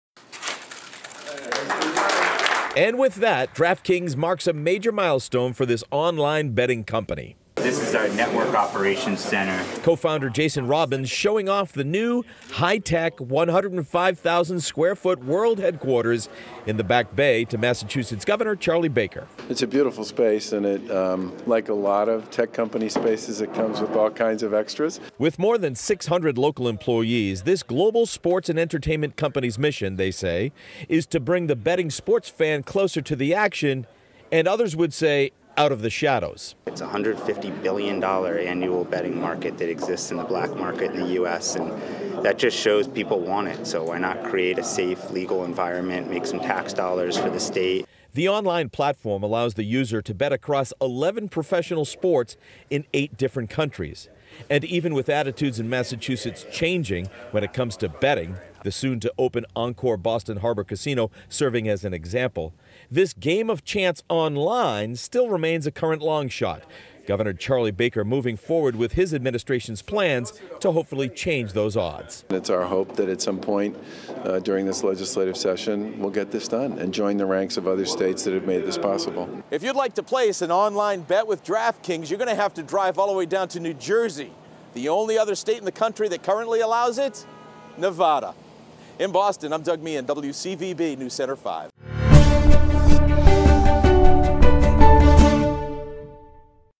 DraftKings opens new headquarters in Boston
The online sports betting company unveiled a 105,000-square-foot facility Tuesday.